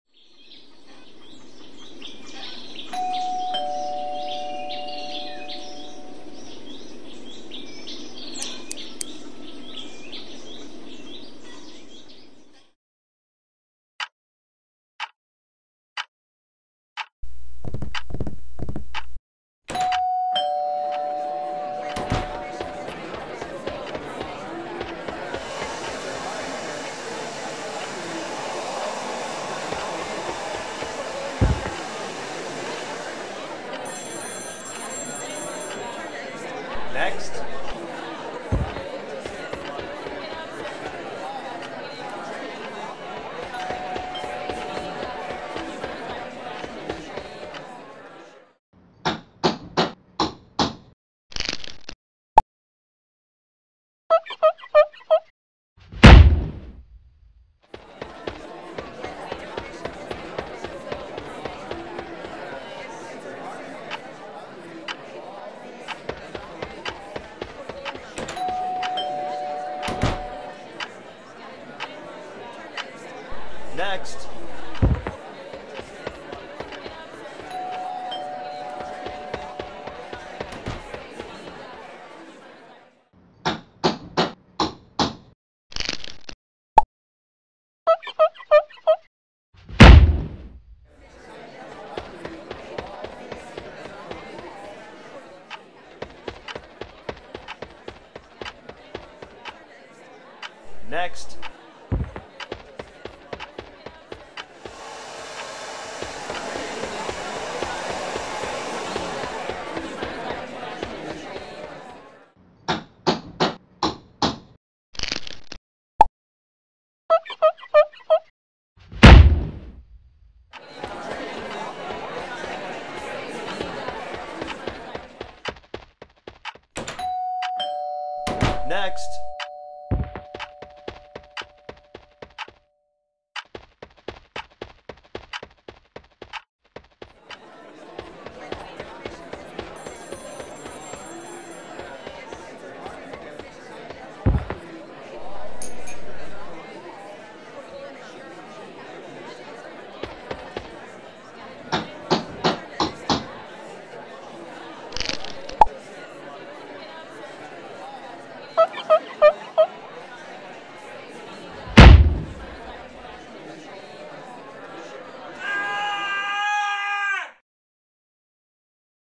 Lego Barber Shop
Everyone needs a haircut eventually. However, Lego haircuts are done a little differently...